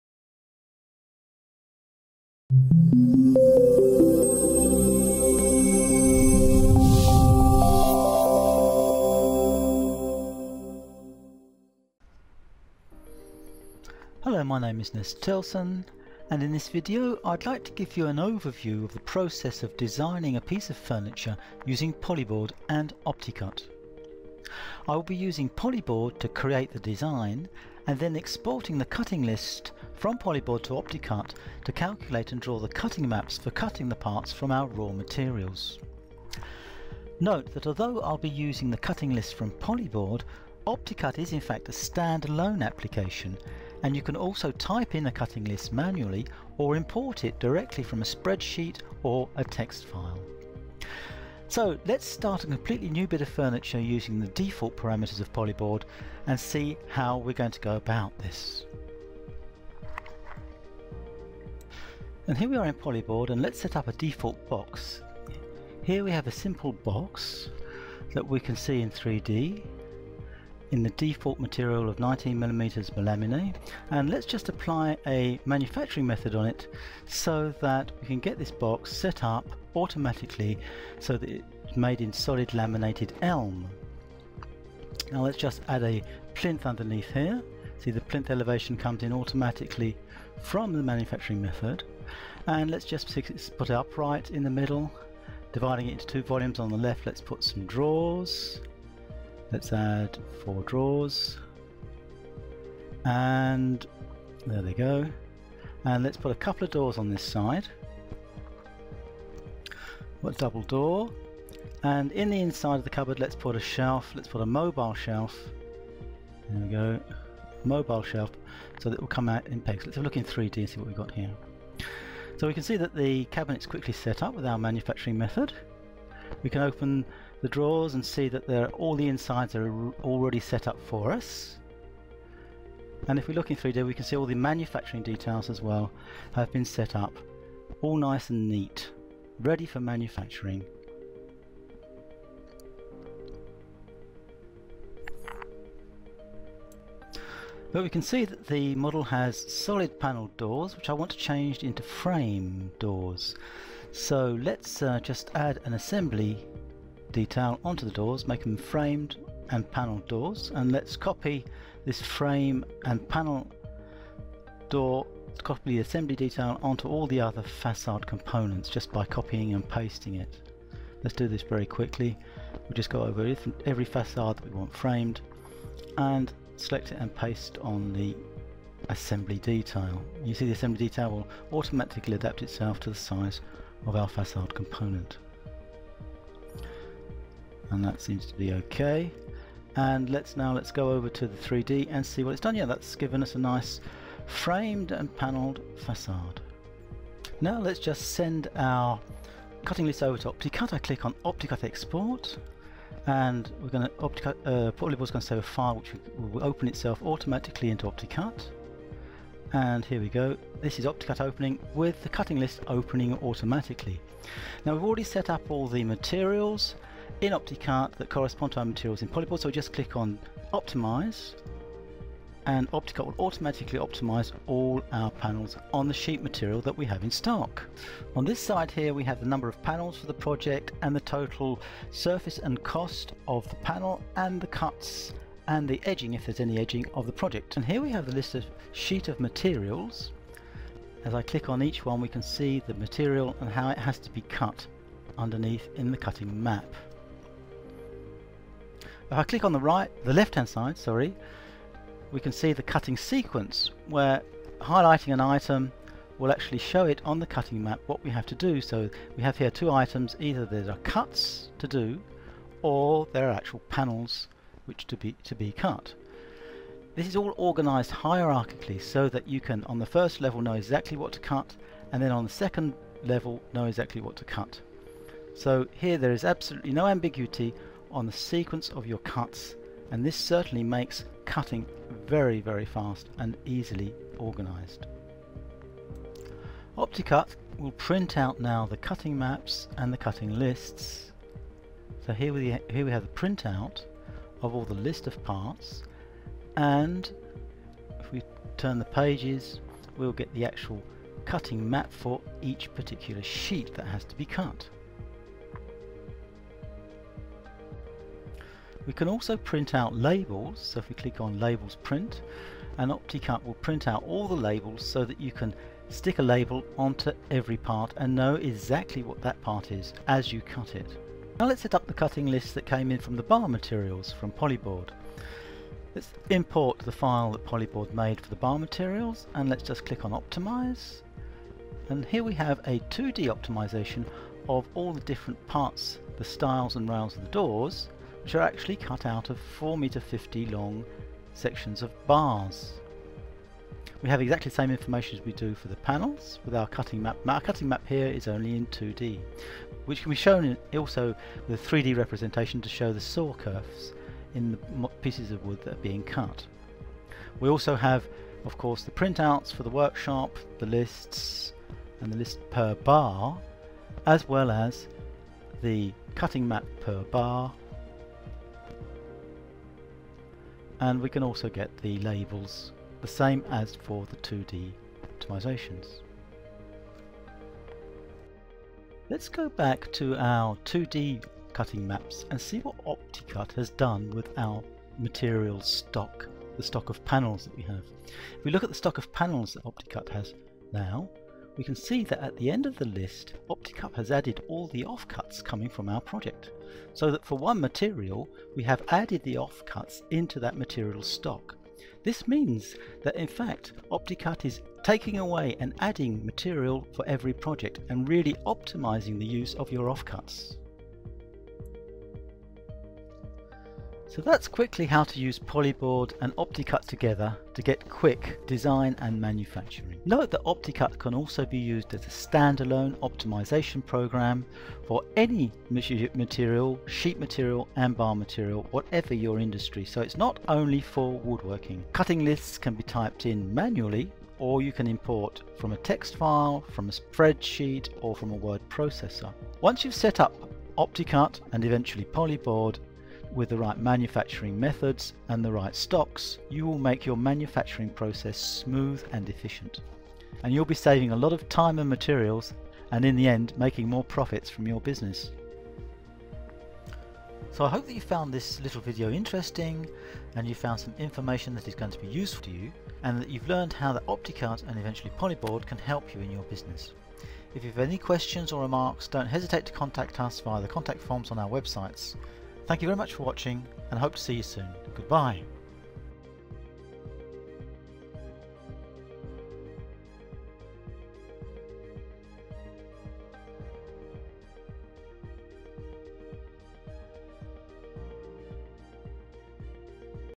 Save Money The Easy Way With OptiCut | Wood Designer Ltd
OptiCut is a cut optimization package that enables the user to fine tune every aspect of the process, and prioritize what important to them, whether it's waste minimization, cutting time, or processing speed.

You also got a built in stock and off cut control system. Each and every time you run a project, all your re-usable off cuts will get added to your inventory ready for use in future projects.

This video also showcases Polyboard, our pro level furniture design and manufacturing software.